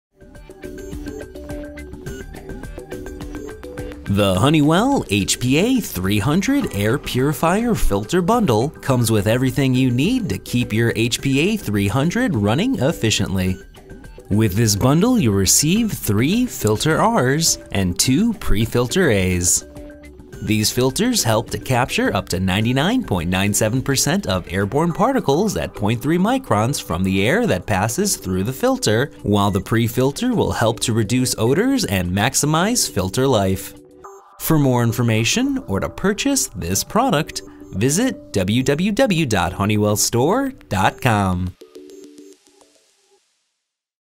Honeywell HPA300 Air Purifier Filter Bundle (3 Filters & 2 Pre-Filters Included)
This bundle includes all filters needed to run your HPA300 air purifier. Included are 3 Honeywell True HEPA Replacement Filter Rs which help capture 99.97% of airborne particles at 0.3 microns from any air passing through the filter. It is highly recommended that you change your filter annually for optimal performance. Establish an unrivaled breathing experience for yourself and your loved ones with Genuine Honeywell Replacement Filters.

This bundle also includes 1 Honeywell Universal Carbon Pre-filter which helps deodorize the air and trap larger particles such as dust, lint, fibers and pet fur. Enjoy the many comforts associated with surrounding yourself with a pristine breathing experience all year round. These Pre-Filters boost and maximize the HEPA's filter life. These Pre-Filters are extremely efficient in blocking bad odors and dust.